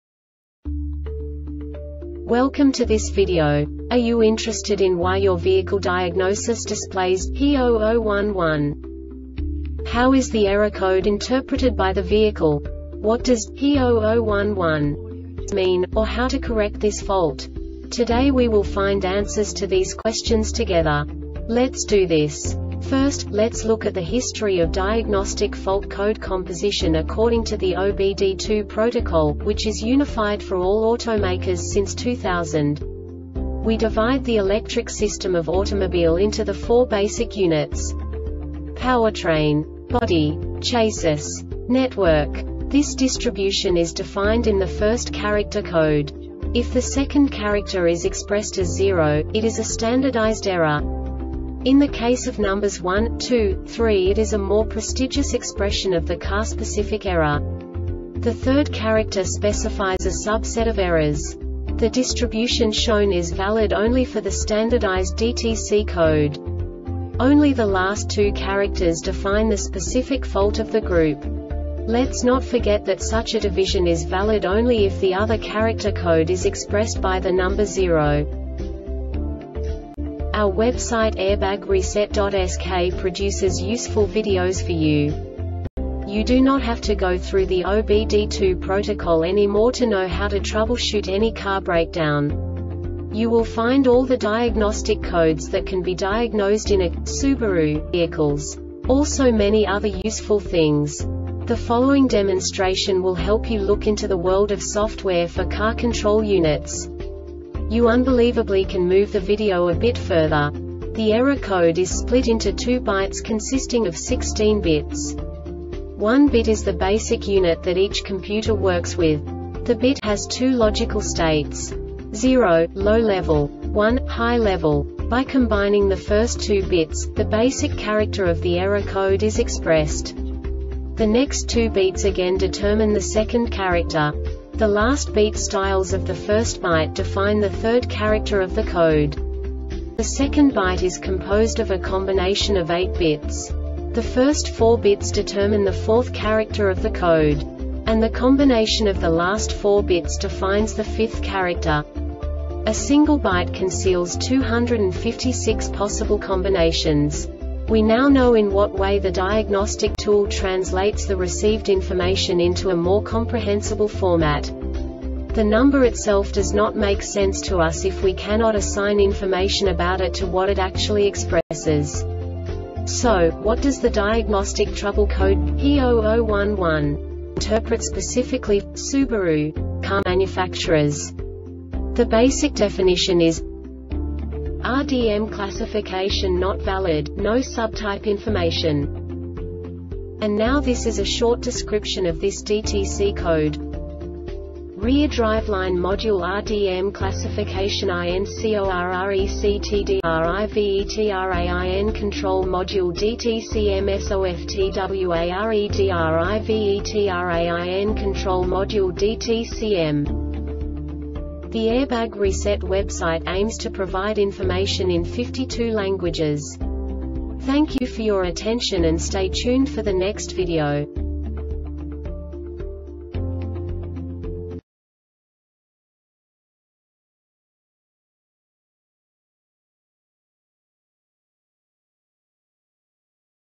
DTC Subaru P0011 Short Explanation
Contents:
0:21 Basic DTC analysis according to OBD2 protocol standard.
1:48 Insight into programming
2:58 A diagnostically understandable description of the Diagnostic Trouble Code
3:05 Basic error definition

Make:
Subaru
Code:
P0011
Definition:
RDM Classification Not Valid - No Subtype Information
Cause:
REAR DRIVELINE MODULE (RDM) CLASSIFICATION INCORRECTDRIVETRAIN CONTROL MODULE (DTCM) SOFTWAREDRIVETRAIN CONTROL MODULE (DTCM)